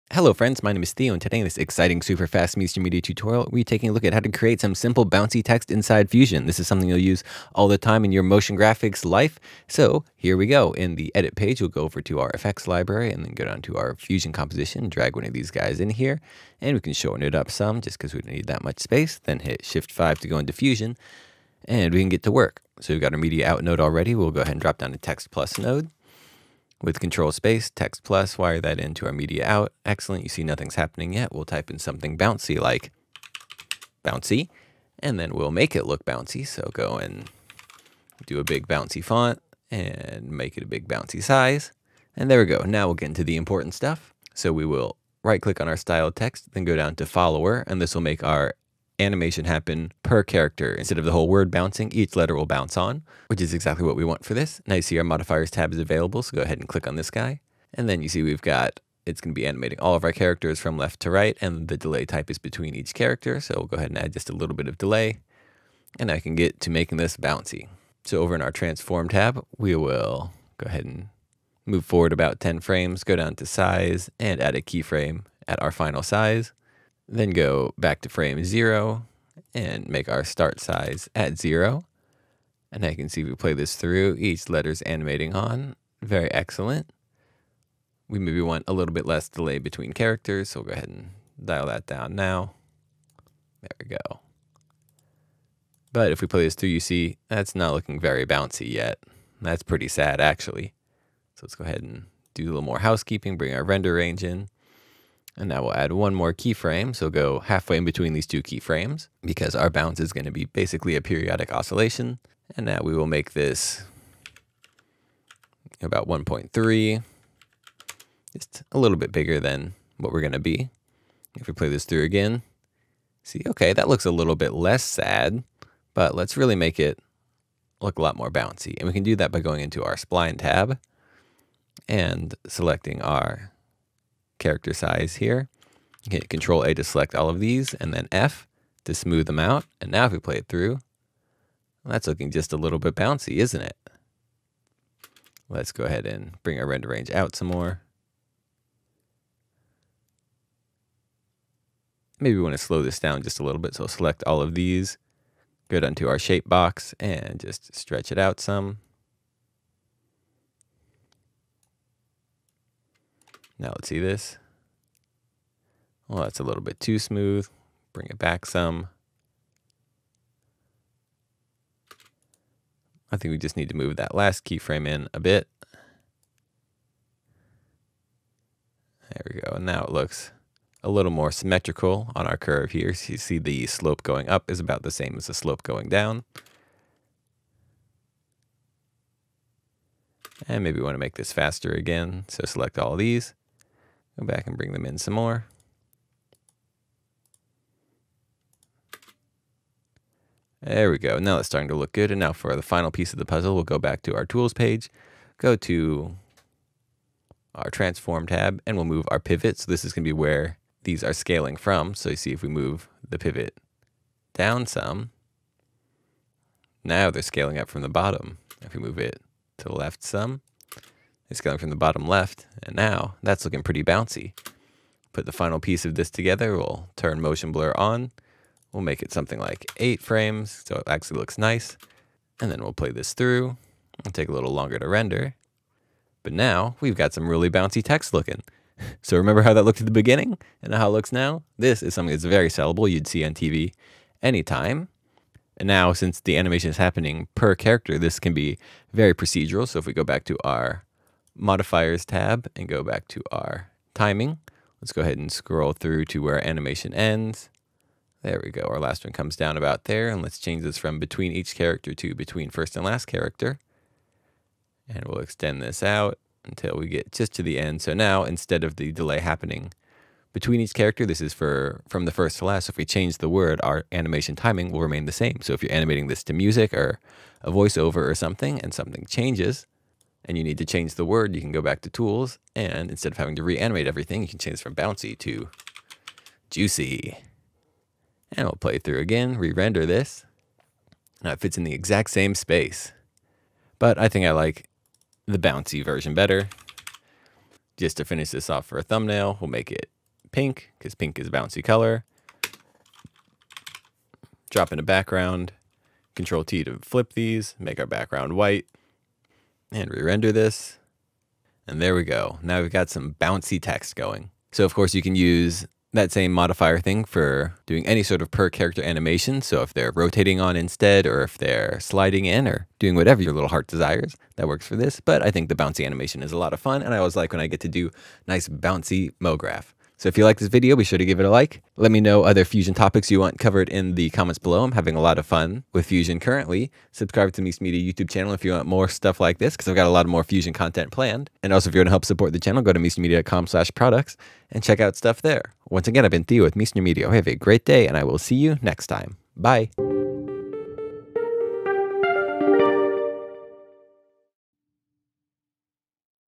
Blackmagic Fusion Basics - Bouncy Text Tutorial
Bouncy, lively text is something that will always be in style, and luckily it is super easy to do in Fusion!